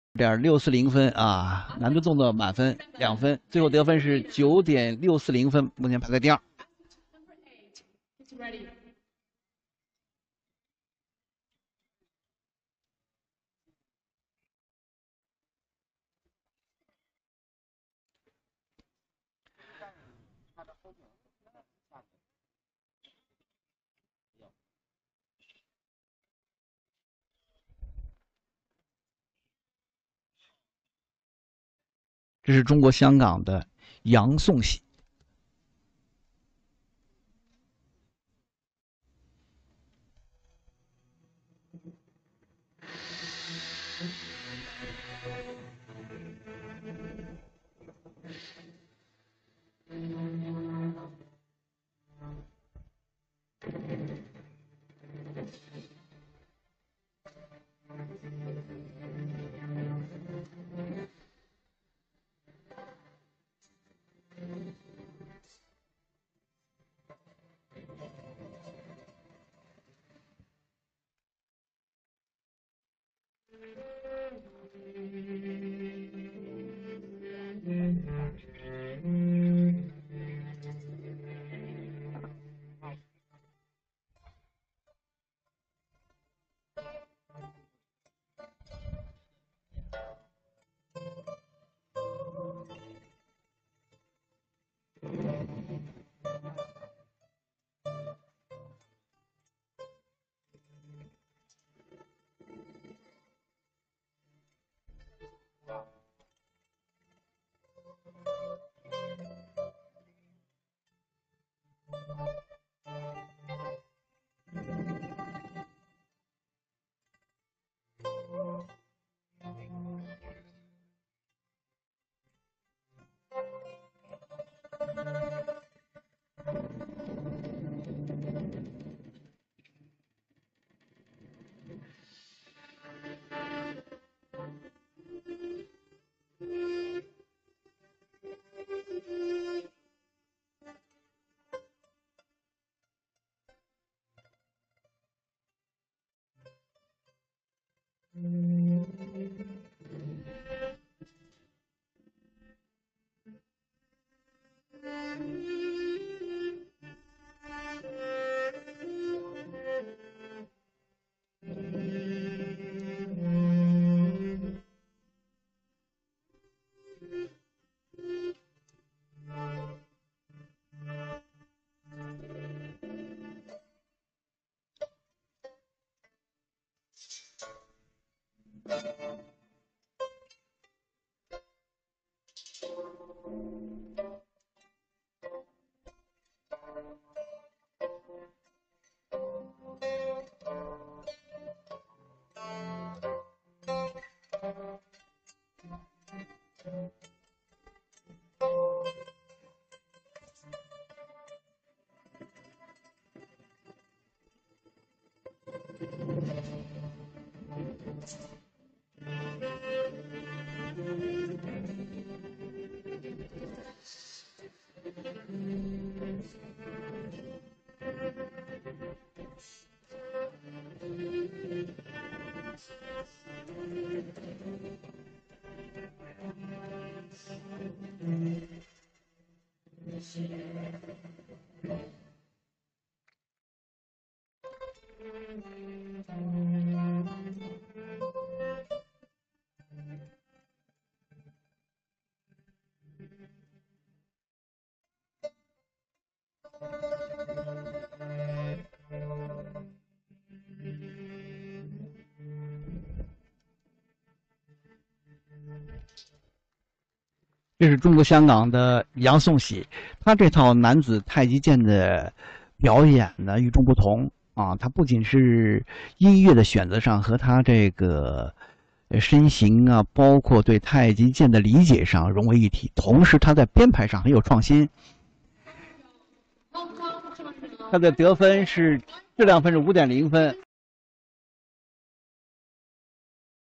Chung Hei Yeung, 2nd Place, Men's Taijijian, IWIT 2024 taijijian kungfu martial arts
Chung Hei Yeung, from HKG, score:9.676, 3rd Place, Men's Taijijian IWIT 2024

International Wushu Invitational Tournament 2024
April 21-26, 2024
Pegasus Watertown, Jiangsu, China